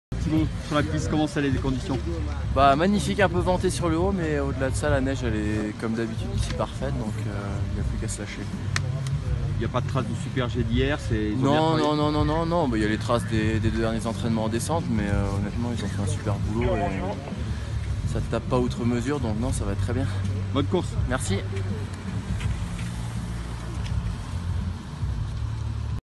Beaver Creek - Réaction Maxence Muzaton
Maxence Muzaton s'exprime à propos de la Birds of Prey avant de remonter prendre le depart de cette descente mythique